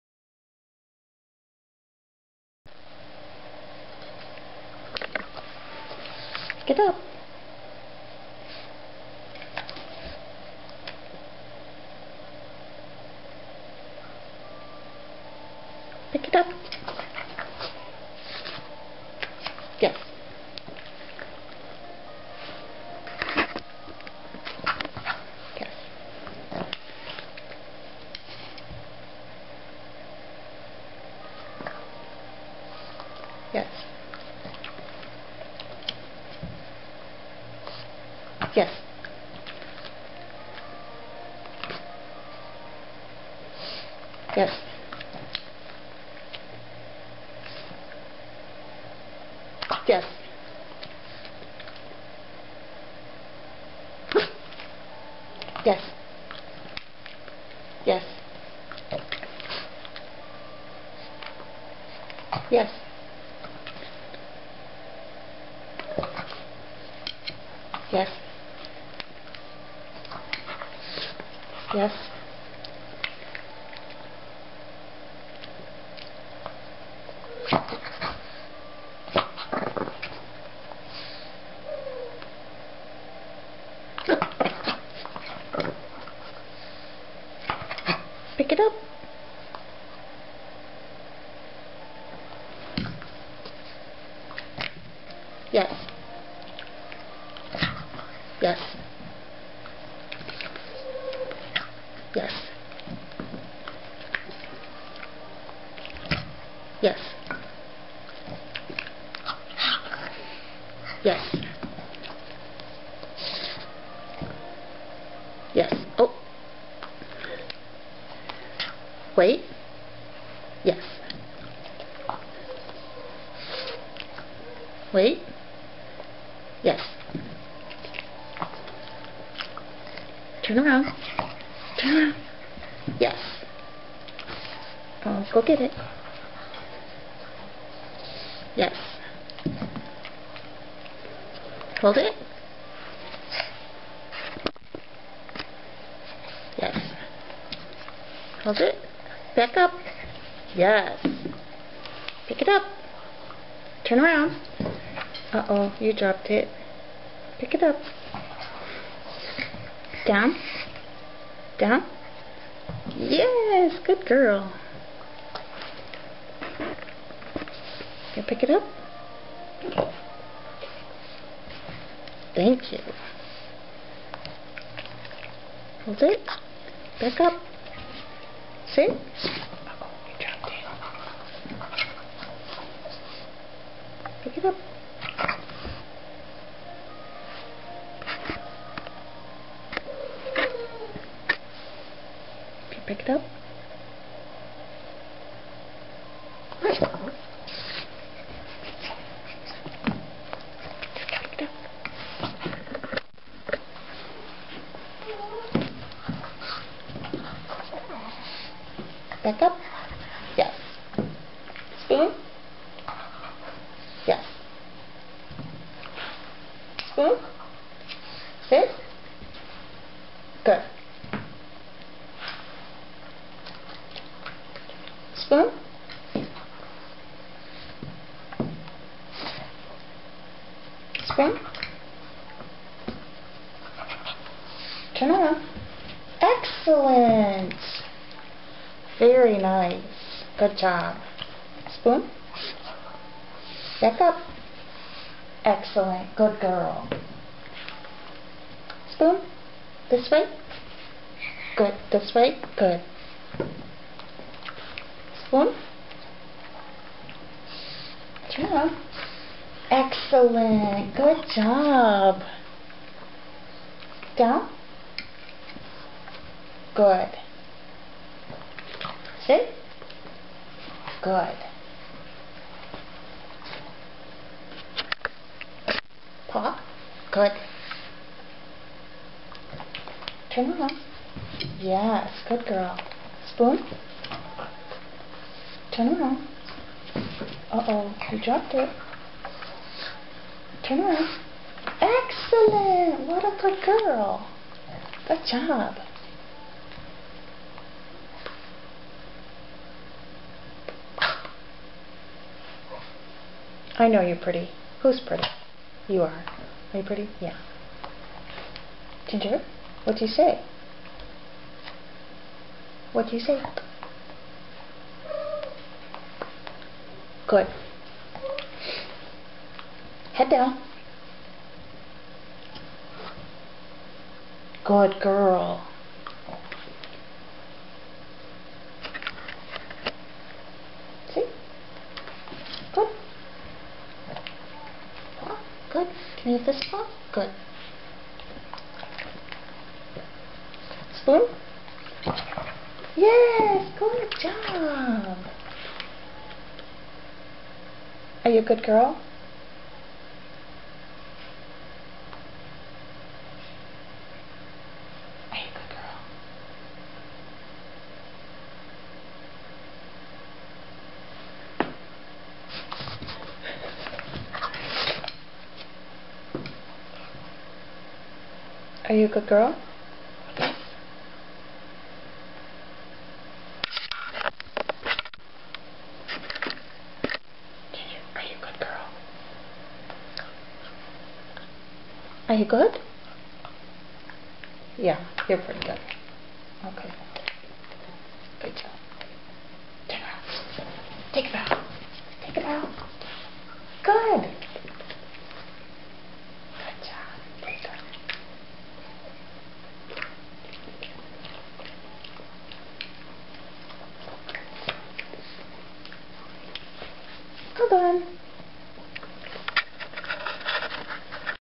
Cabin Fever - A Training Session with Ginger the Pug.wmv
Some fun stuff to do to break up boredom when it's too icky to go outdoors. Here we work on teaching Ginger to hold an object in her teeth while running through basic behaviors.

I am using clicker training here, but using "yes" or "good" as a marker. Even though I'm inconsistent and use both words, Ginger is savvy enough to understand anyway.